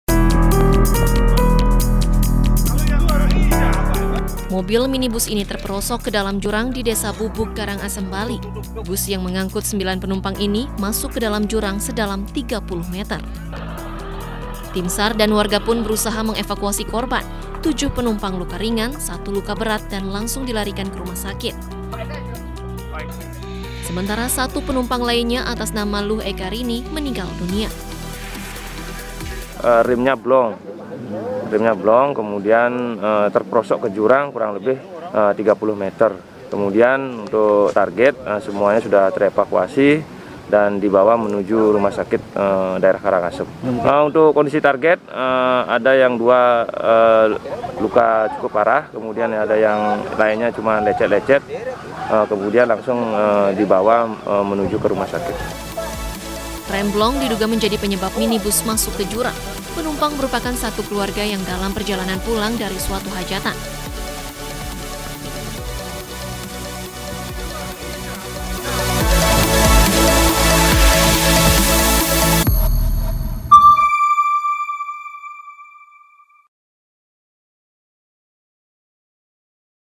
Diduga Rem Blong, Sebuah Minibus di Karangasem Masuk Jurang Sedalam 30 Meter
Sebuah minibus yang membawa sembilan penumpang, masuk ke dalam jurang sedalam 30 meter di Desa Bubug di Karangasem, Bali. Tim SAR dan warga berusaha mengevakuasi korban.

Android: inews.id/android
iPhone: inews.id/ios